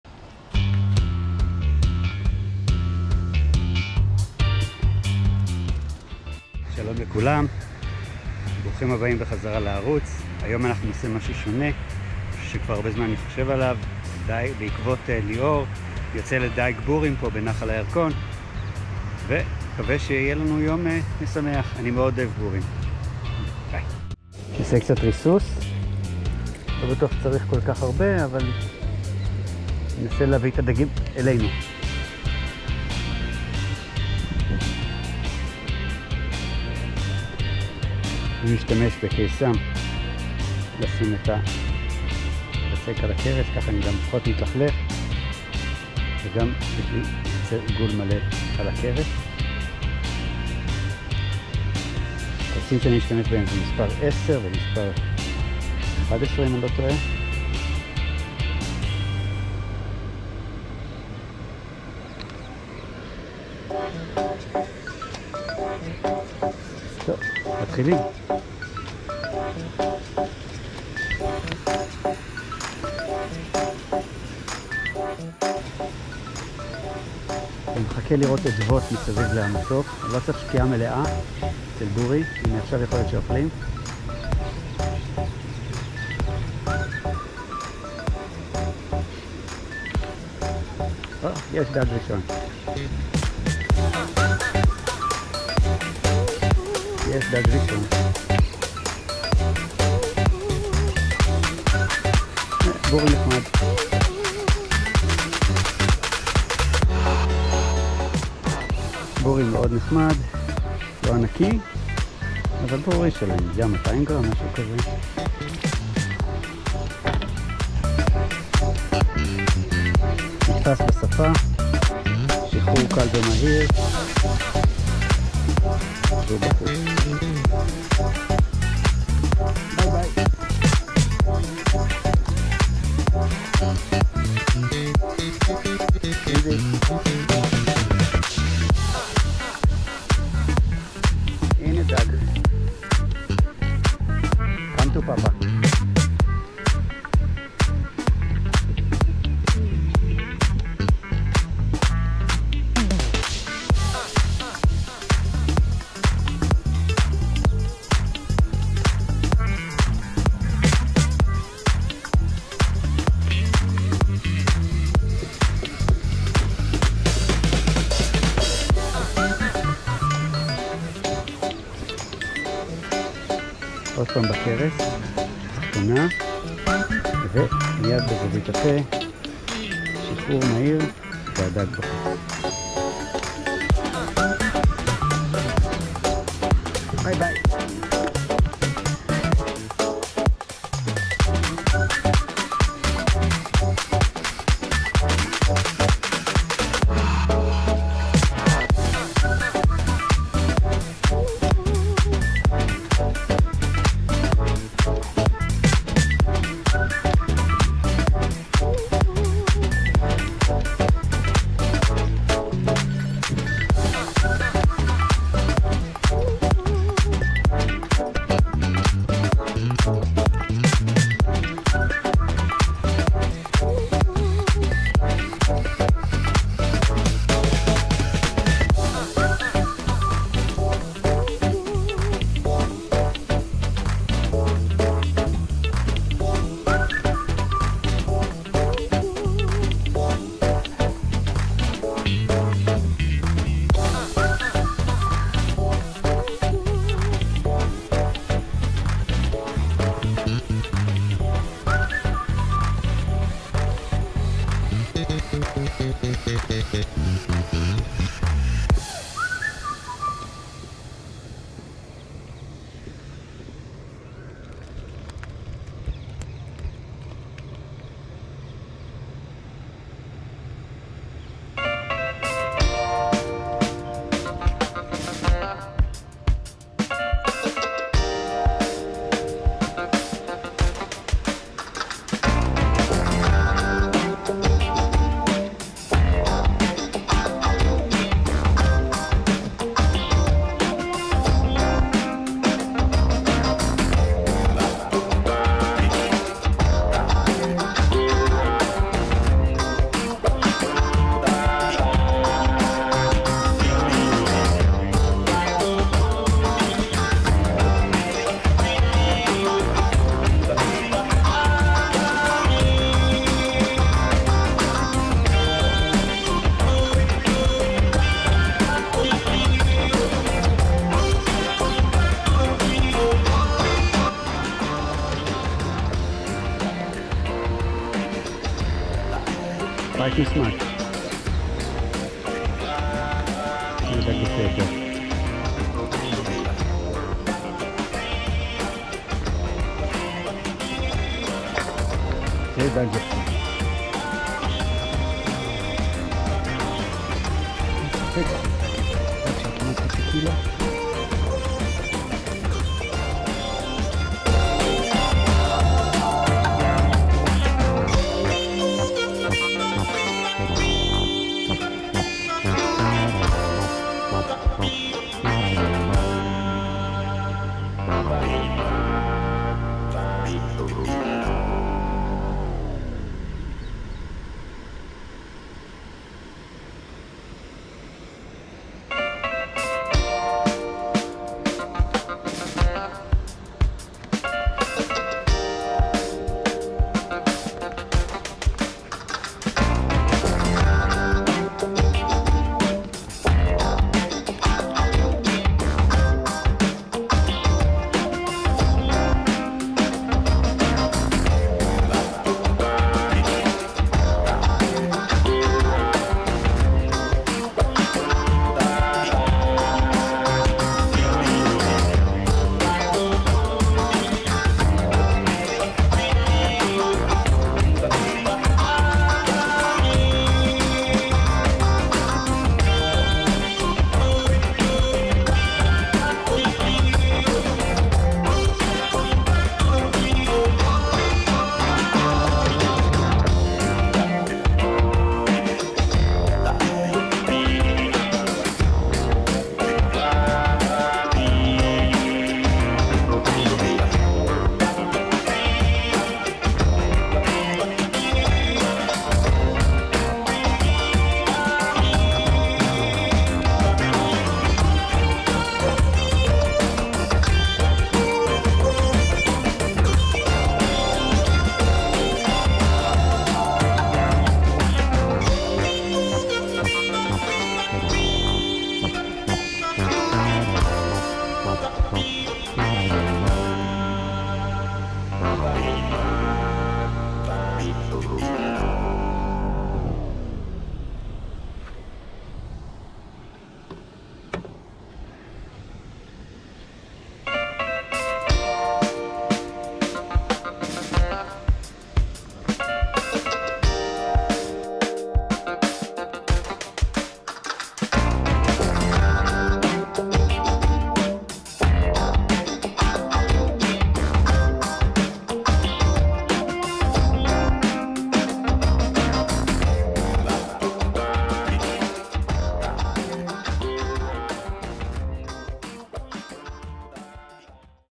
Mullets fun on pole - Yarkon river July 1, 2020
Fishing for Mullets is a long love story for me.
I stated fishing for this eluding species as a youngster, and I still enjoy doing this today.

I fished on the Yarkon river in Tel-Aviv area.
Unfortunately, the water are polluted and therefore I can only C&R.

I caught around 15 fish, the biggest one was around 1/2 kg (a pound ~).

My Tackle:
Pole:          Meteor Carbon 6m
Main line:  0.20 mm nylon
Float:         2-3 g
Weight:     1-2 g
Hooks:      size 8 & 10
Bait:           Sticky dough made of wheat flour, sugar and water